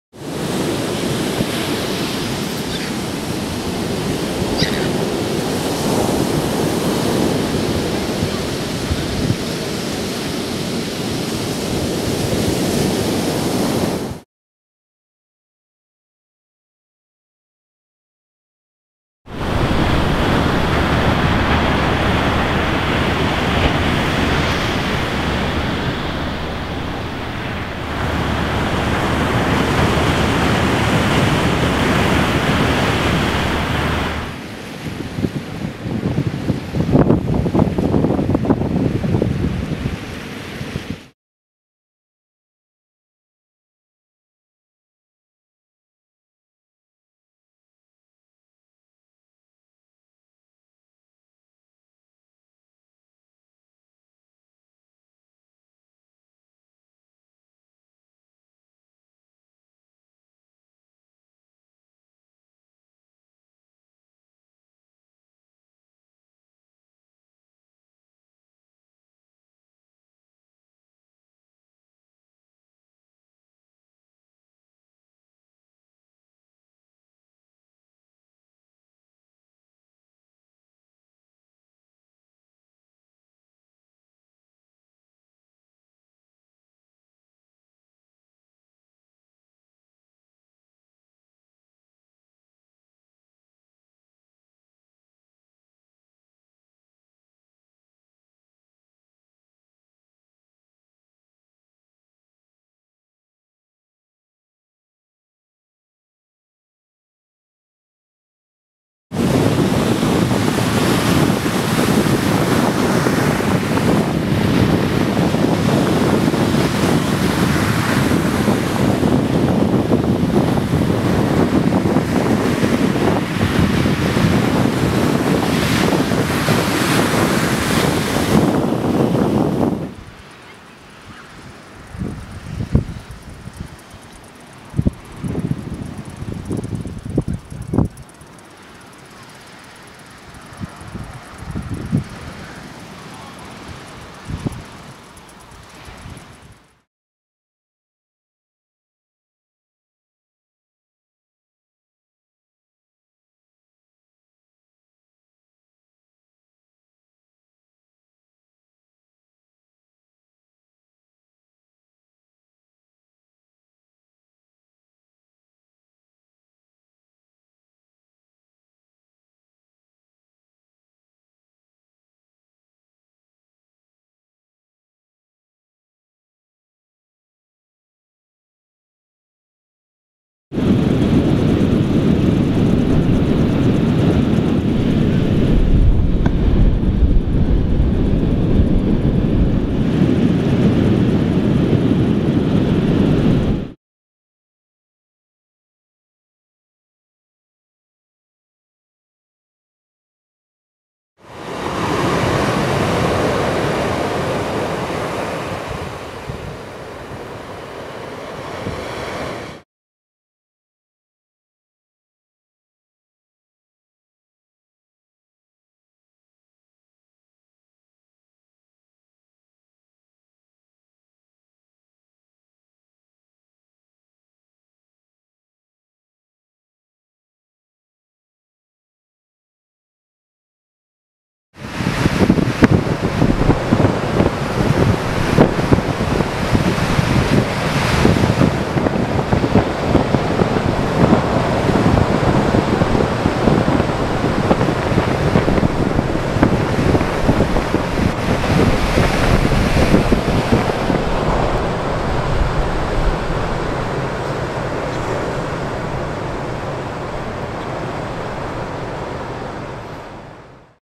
Hoodie movie
A snapshot into the lives of Hooded Plovers "Hoodies" on Australian beaches.